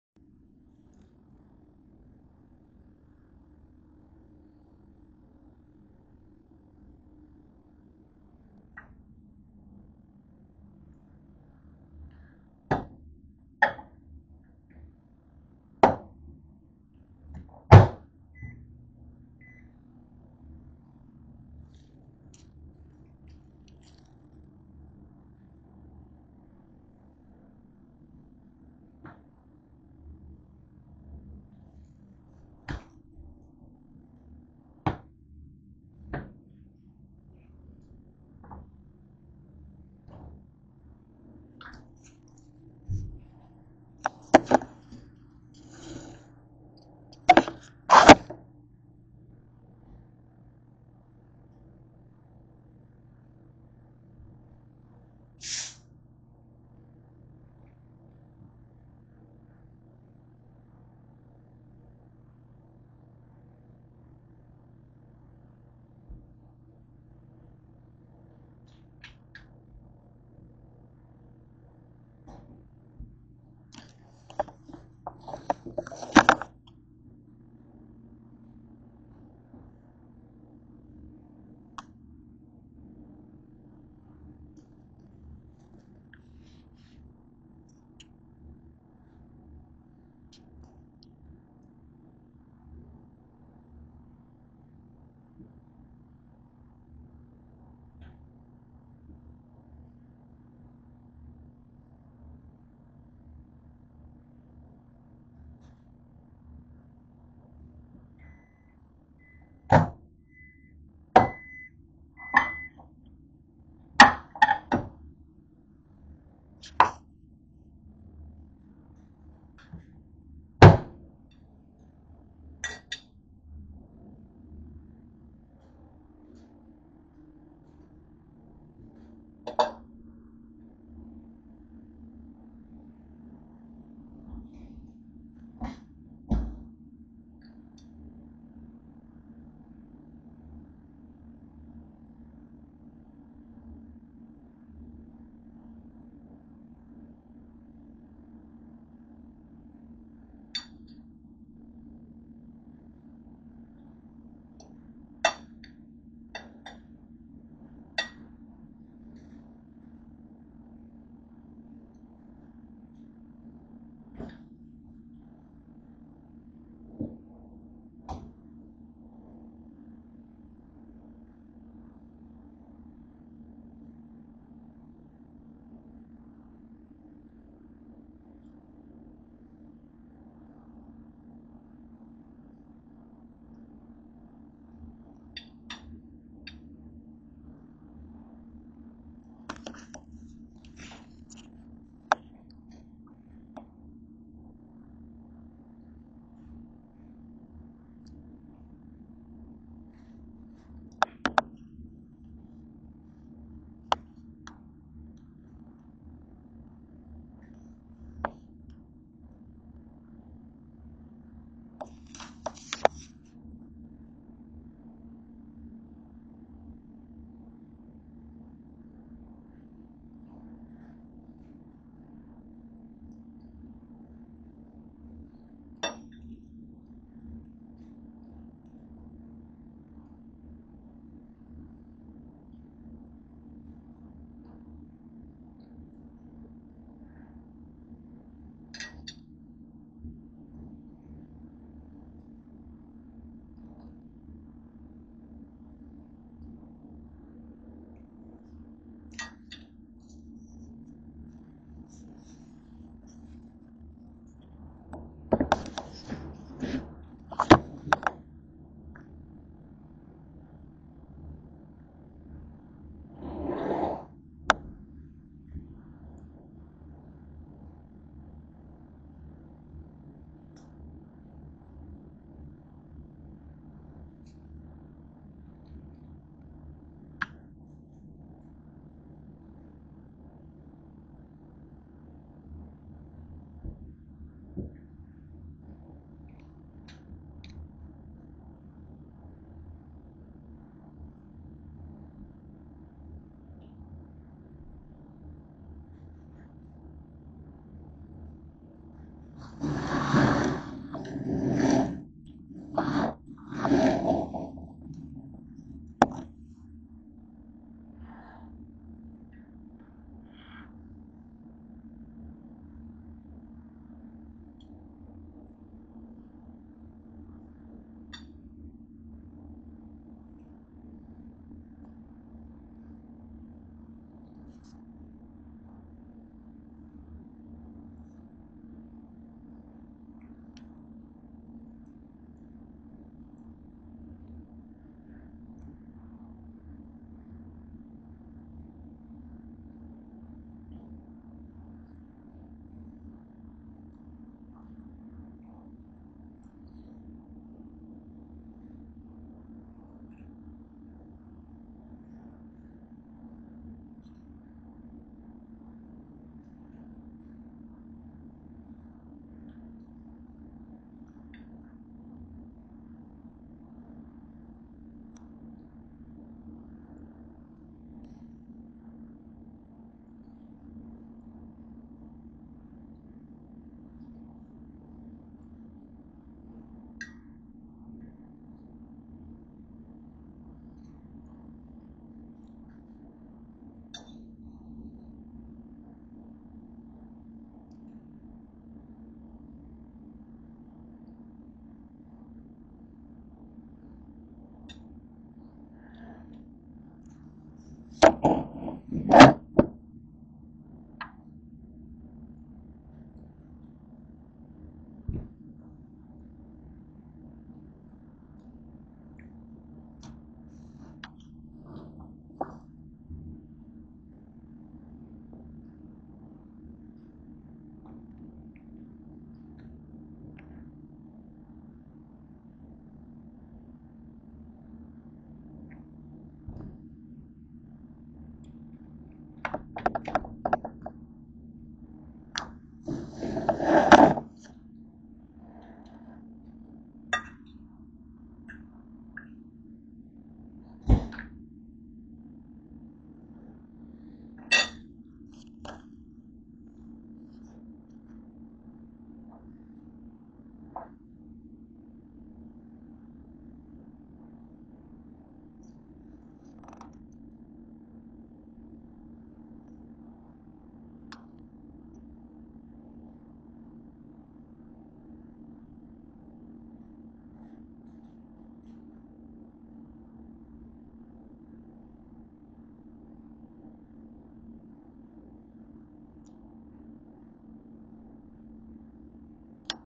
A couple of Bee & Bean Burritos with Winco Brand String Cheese melted on Top; Thursday Aug. 22nd 24
So, I microwaved a couple of Beef & Bean El Monterey Burritos with some Winco Brand String Cheese on top, and had some Diet Coke to Drink XD